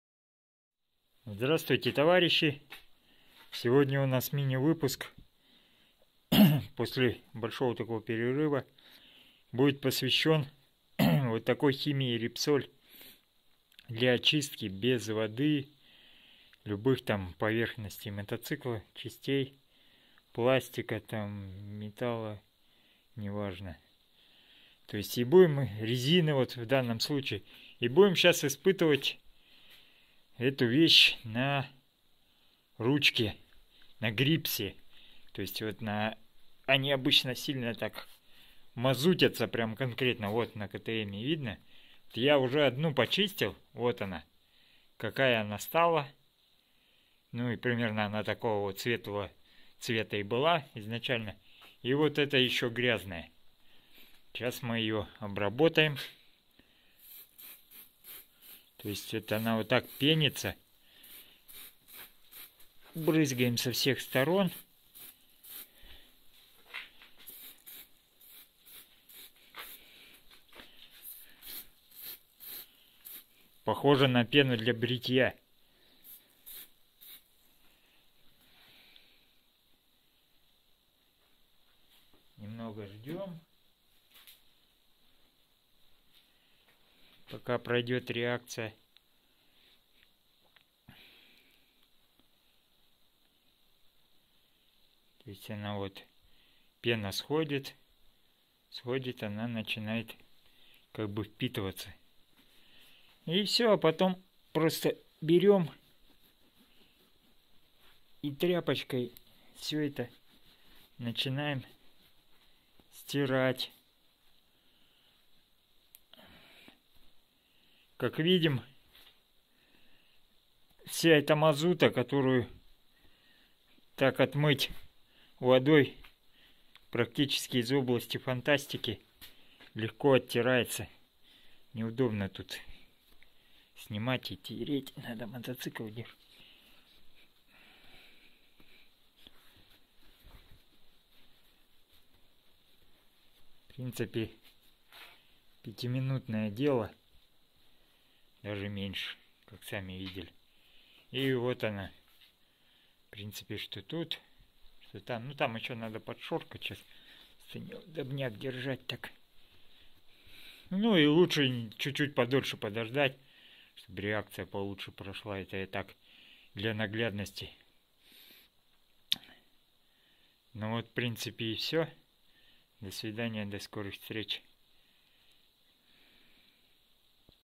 "Излучатель МотоБокса" испытание химии Репсол для очистки поверхностей мототехники.
Средство для очистки металлических, стекловолокнистых и пластиковых деталей мотоцикла.
Удаляет загрязнения, жир и остатки насекомых без использования воды.
Полирует и защищает хромированные детали, окрашенные и пластиковые поверхности и не повреждает их благодаря специальному составу.